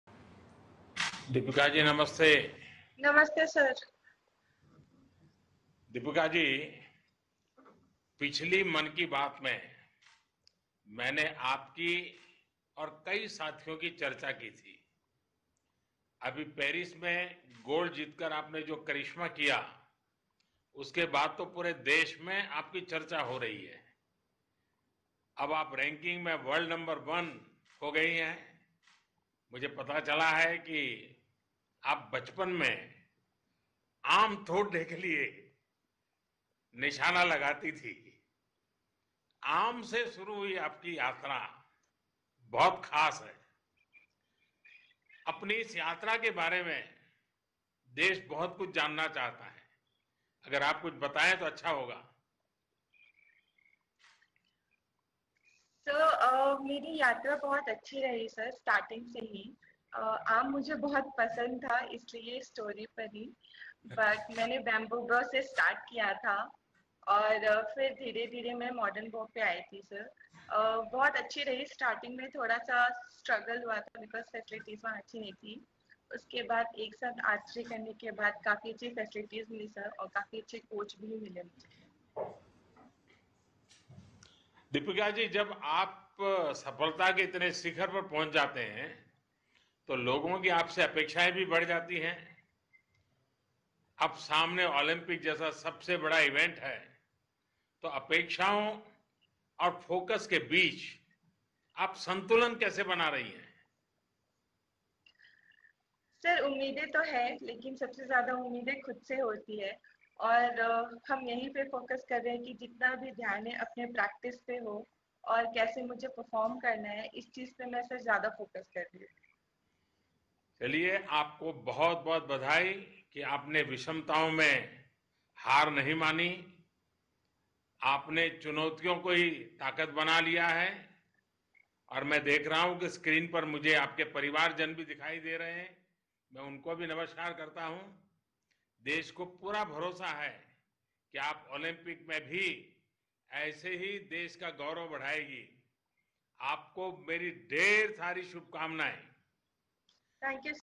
PM Shri Narendra Modi interacts with Deepika Kumari
PM Modi asks Deepika Kumari about plucking mangoes in her childhood